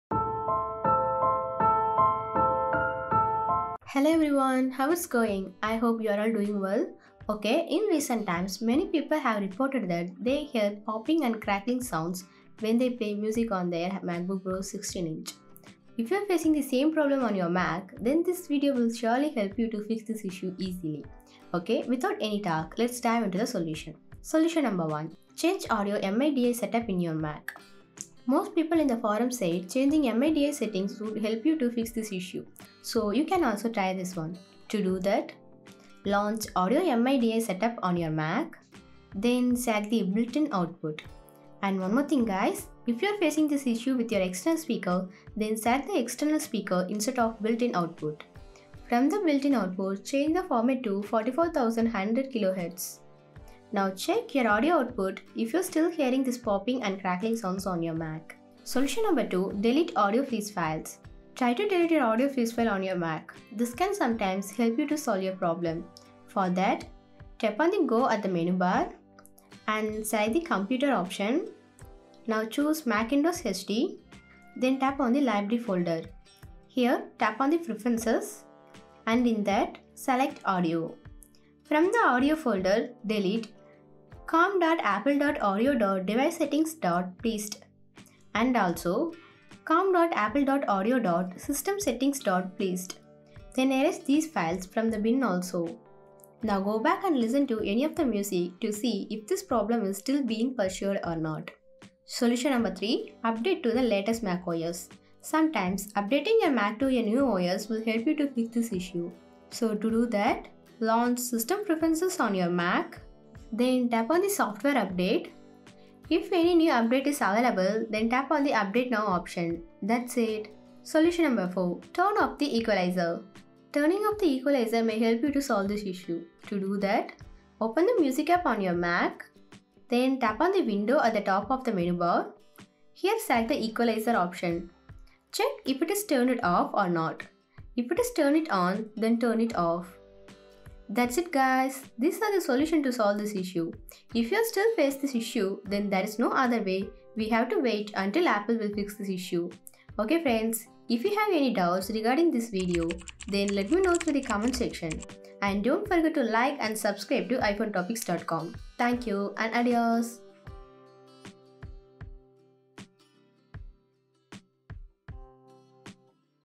Fixed: MacBook Pro 16" M1 2021 Popping Sound When Music Playing
Hello, folks. In this video, we are going to see "How to fix Macbook pro 16" M1 2021 popping sound when music plays". I hope this really helps you solve this issue.
⏳Timstamp:
0:00  Introduction
0:24  Solution 1: Change Audio MIDI Setup in your Mac
0:58  Solution 2: Delete Audio Plist Files
1:41  Solution 3: Update to the latest MacOS
2:01  Solution 4: Turn Off the Equalizer
2:20  Thanks Note